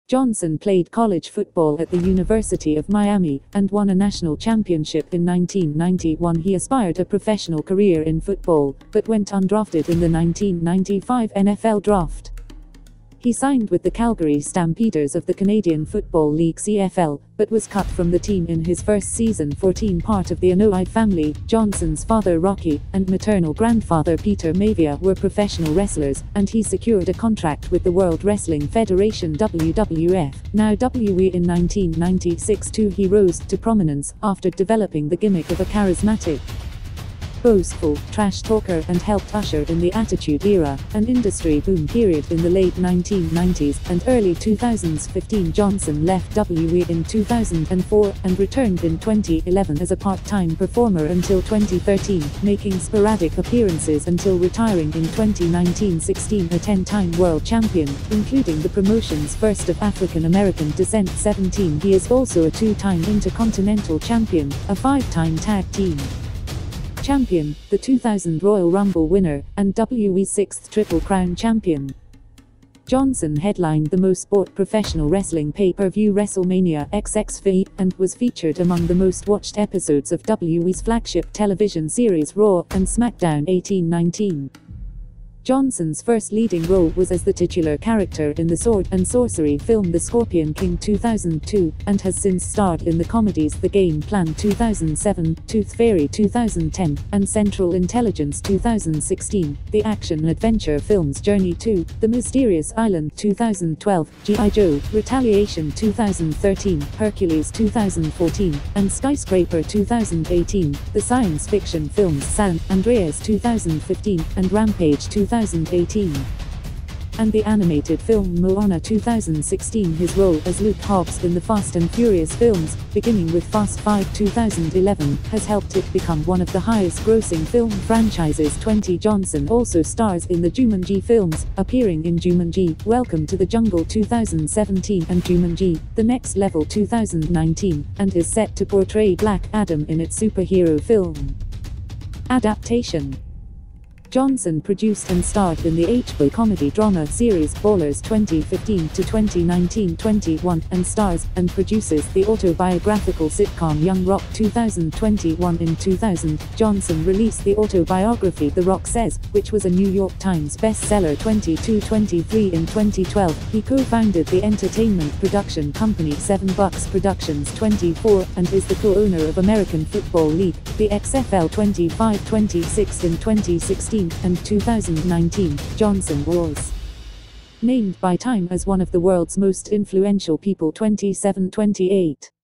THE ROCK
Johnson played college football at the University of Miami, and won a national championship in 1991. He aspired a professional career in football, but went undrafted in the 1995 NFL Draft. He signed with the Calgary Stampeders of the Canadian Football League (CFL), but was cut from the team in his first season.[14] Part of the Anoa'i family, Johnson's father Rocky and maternal grandfather Peter Maivia were professional wrestlers, and he secured a contract with the World Wrestling Federation (WWF, now WWE) in 1996.[2] He rose to prominence after developing the gimmick of a charismatic, boastful, trash-talker and helped usher in the Attitude Era, an industry boom period in the late 1990s and early 2000s.[15] Johnson left WWE in 2004 and returned in 2011 as a part-time performer until 2013, making sporadic appearances until retiring in 2019.[16] A 10-time world champion, including the promotion's first of African-American descent,[17] he is also a two-time Intercontinental Champion, a five-time Tag Team Champion, the 2000 Royal Rumble winner, and WWE's sixth Triple Crown champion. Johnson headlined the most-bought professional wrestling pay-per-view (WrestleMania XXVIII) and was featured among the most watched episodes of WWE's flagship television series (Raw and SmackDown).[18][19]
Johnson's first leading role was as the titular character in the sword and sorcery film The Scorpion King (2002), and has since starred in the comedies The Game Plan (2007), Tooth Fairy (2010), and Central Intelligence (2016); the action-adventure films Journey 2: The Mysterious Island (2012), G.I. Joe: Retaliation (2013), Hercules (2014), and Skyscraper (2018); the science-fiction films San Andreas (2015) and Rampage (2018), and the animated film Moana (2016). His role as Luke Hobbs in the Fast & Furious films, beginning with Fast Five (2011), has helped it become one of the highest-grossing film franchises.[20] Johnson also stars in the Jumanji films, appearing in Jumanji: Welcome to the Jungle (2017) and Jumanji: The Next Level (2019), and is set to portray Black Adam in its superhero film adaptation.
Johnson produced and starred in the HBO comedy-drama series Ballers (2015–2019),[21] and stars and produces the autobiographical sitcom Young Rock (2021). In 2000, Johnson released the autobiography The Rock Says, which was a New York Times bestseller.[22][23] In 2012, he co-founded the entertainment production company Seven Bucks Productions,[24] and is the co-owner of American football league, the XFL.[25][26] In 2016 and 2019, Johnson was named by Time as one of the world's most influential people.[27][28]